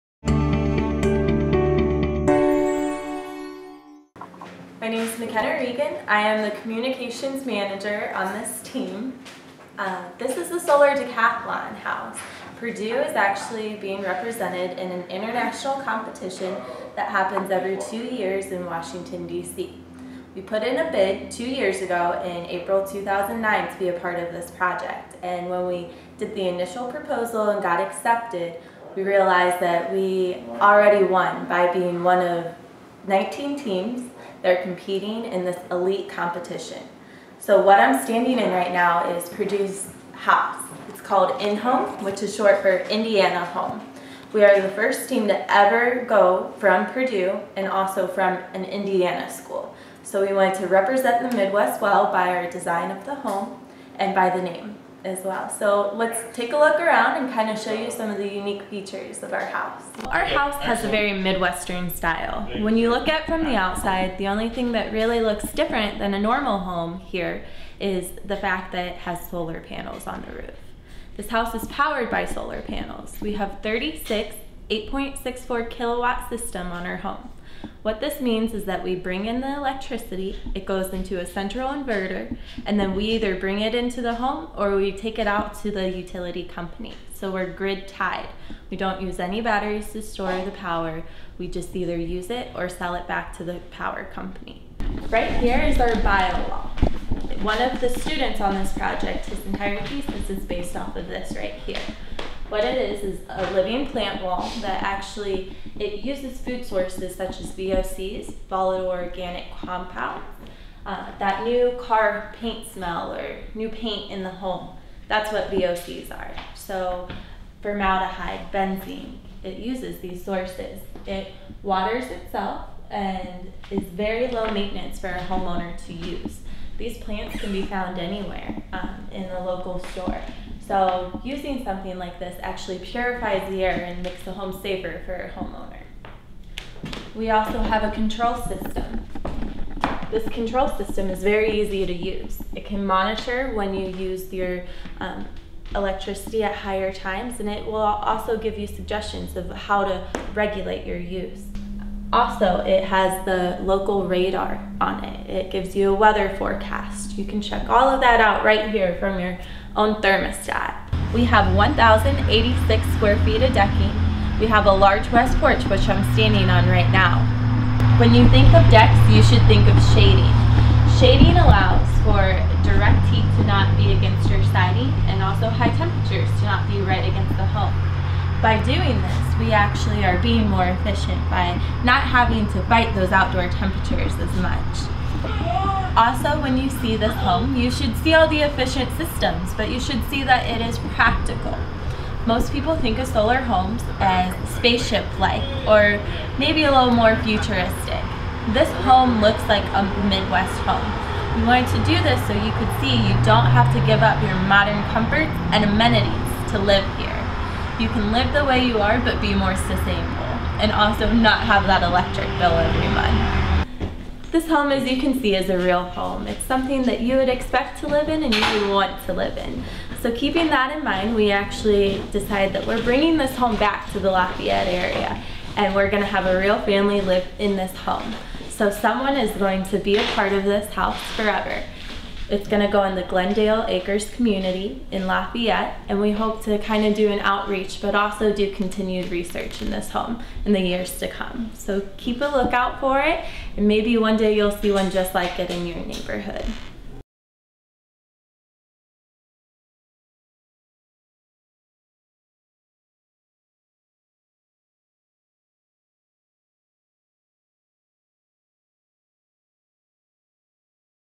The Purdue Exponent-Purdue INhome
Take a tour of the home the Purdue Solar Decathlon team built in for the U.S. Department of Energy Solar Decathlon competition.